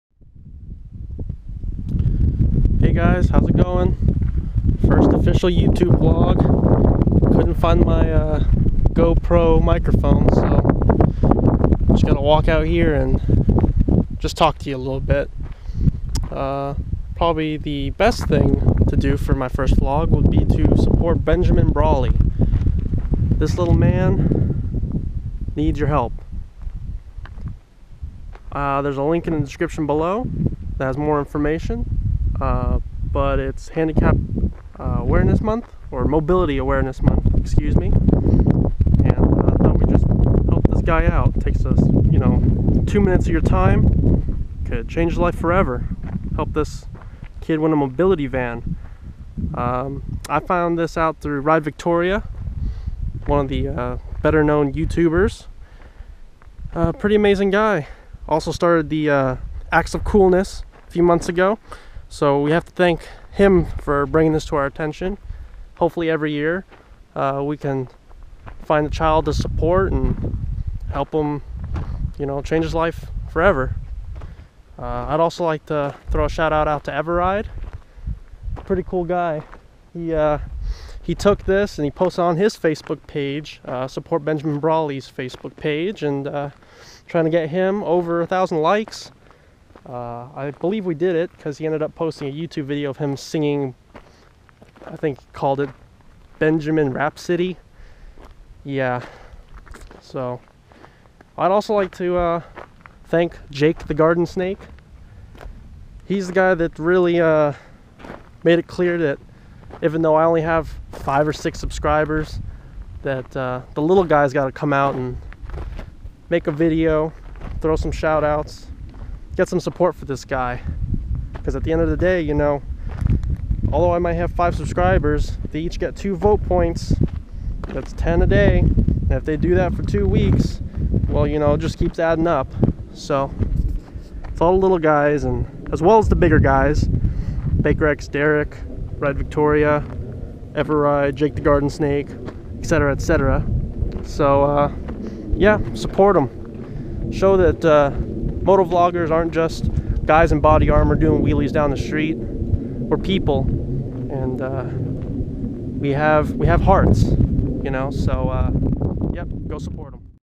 VOTE FOR BENJAMIN BRAWLEY
Vote for Benjamin. What we take for granted; walking, driving to the store, going to the park, is a huge challenge for the Brawley Family. Benjamin, being handicap, requires a wheelchair. Going to the store to get milk is no easy task. Help ease his families struggles, and add some comfort to their life. It require nothing but a few minutes of your time, to change someone's life. Show the world that people still care. Vote for Ben to help him win a mobility van.